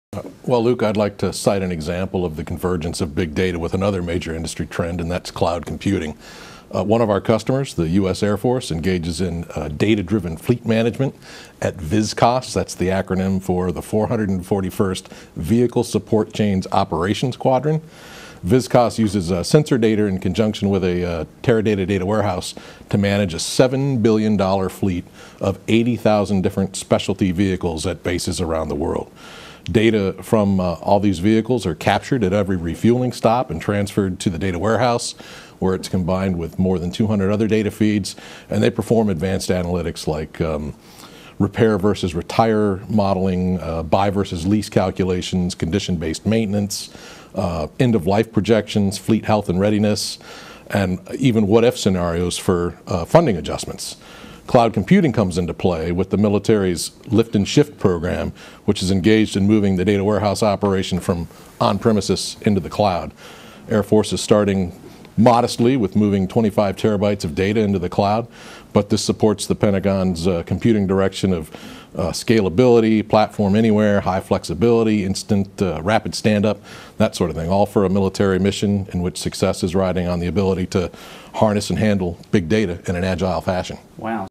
FEF Big Data Progress Teradata May 2018
Teradata's Alan Ford on Progress Made with Big Data Programs in Government-Federal Executive Forum (May 2018)